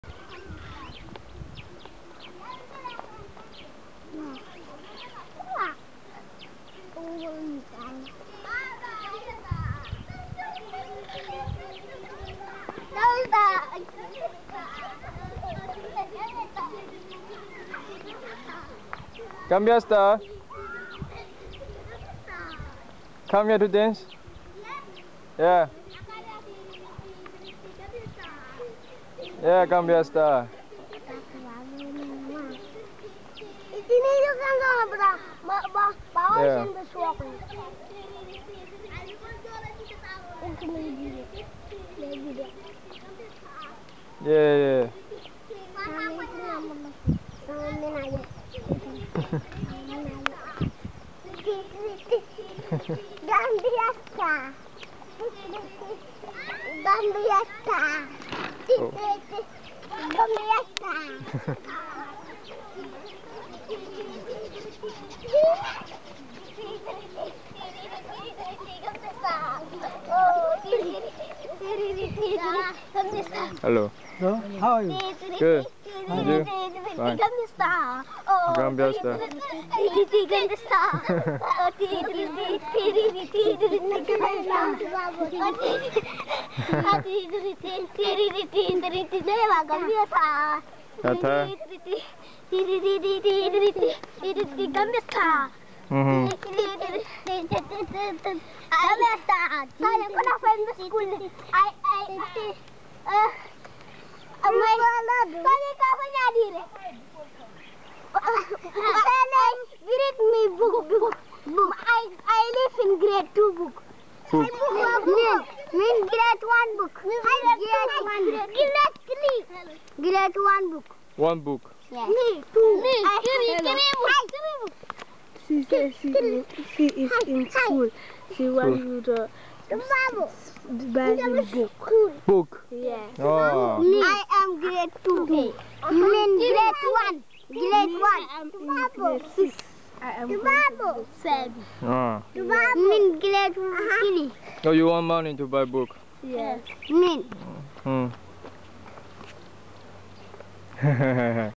Gambia Star Girl 甘比亞之星女孩 - $$ # 2/2
位於甘比亞的Janjanbureh島上，女孩們已經說bye bye, 看我還在拍就繼續跳著，後來圍籬內的母親指示他們要買書的錢，我苦笑之後關了攝影，給了他們一些零錢。